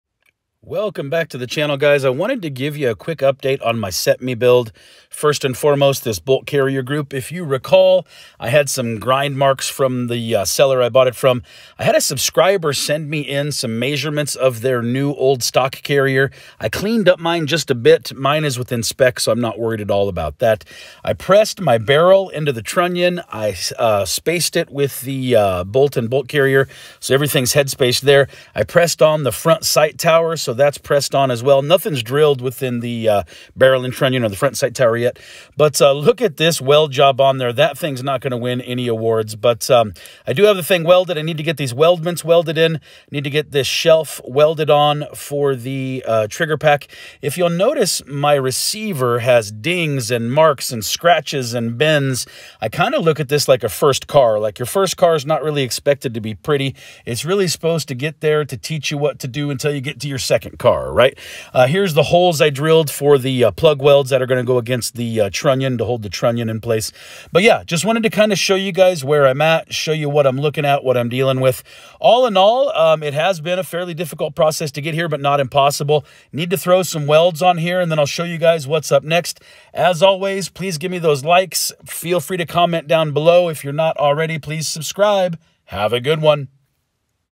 BATTLE RIFLE Build Series - CETME C Build Update
Quick update on my Cetme build.

BCG was lightly sanded at the tip to remove grind marks.  Length was checked and determined to be within spec.

Barrel pressed into trunnion and headspaced, front sight tower pressed on.  Neither of these are drilled or pinned yet in case I need to fine tune anything.

Receiver has taken some abuse but is going to hold up just fine.  She won’t be pretty, but it will be a workhorse.